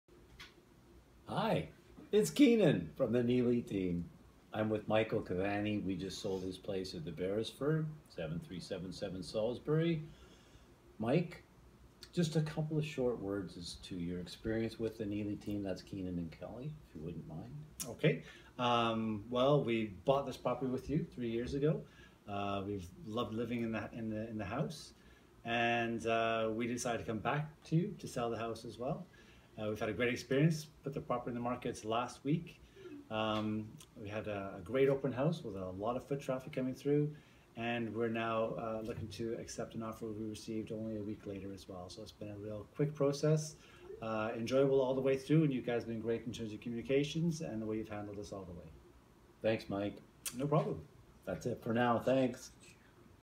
Michael and Claire Testimonial
Michael gives his testimonial after we successfully sold his condo in multiple offers for over the asking price